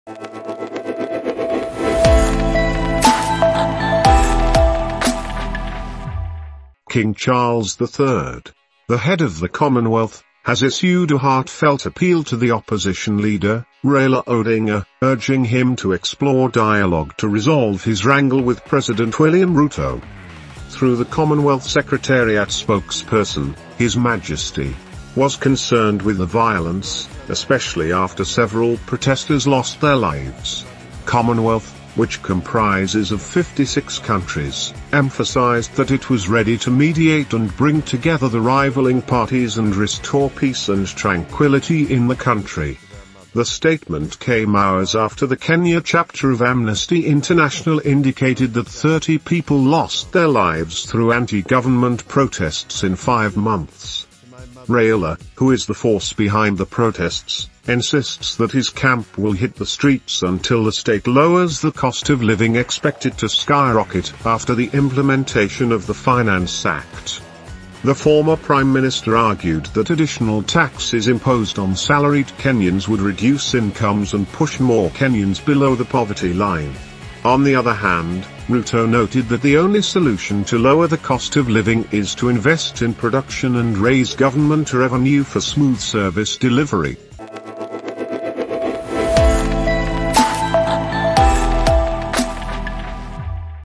King Charles III Special appeal to Raila Odinga On Anti Government Protest ➤ News54.
King Charles III has issued a heartfelt appeal to opposition leader, Raila Odinga

► The Official news54 Channel, Catch up with up-to-the-minute news and information from around the world.

©news54.2023